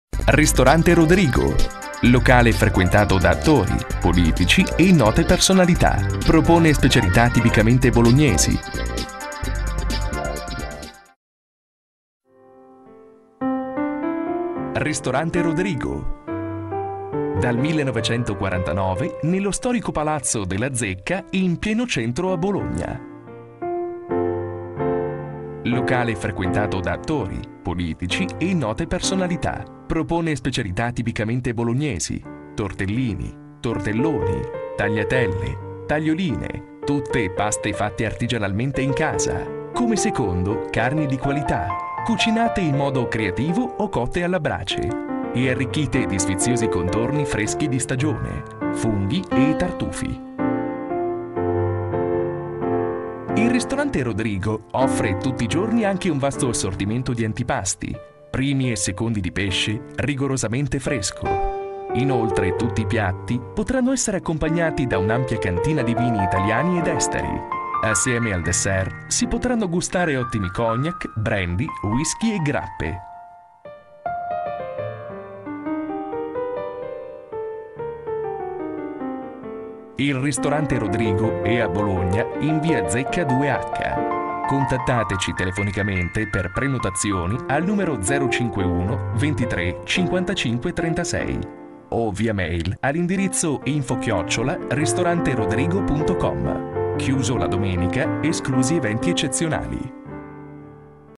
RISTORANTE RODRIGO BOLOGNA (BOLOGNA)
RISTORANTE RODRIGO 2/H, v. Zecca Tel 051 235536, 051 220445 Fax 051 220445 Dal 1949, nello storico palazzo della Zecca, si trova il ristorante Rodrigo, in pieno centro, a 100 metri da p. Maggiore. Il locale è frequentato da attori, politici, industriali e note personalità. Le specialità, tipicamente bolognesi sono: tortellini, tortelloni, tagliatelle, tagliolini, passatelli in brodo, pasta e fagioli; tutte paste fatte artigianalmente in casa, oltre a quelle secche con vari condimenti. Come secondi, il ristorante dispone di tante qualità di carni elaborate o alla brace, con funghi e tartufi freschi in stagione. Vasto assortimento di pesce fresco tutti i giorni, dagli antipasti, alla pasta, ai secondi. Come dessert, frutta e dolci secondo le stagioni. Vastissima enoteca di vini italiani ed esteri; grandissimo assortimento di cognac, brandy, whisky e grappe.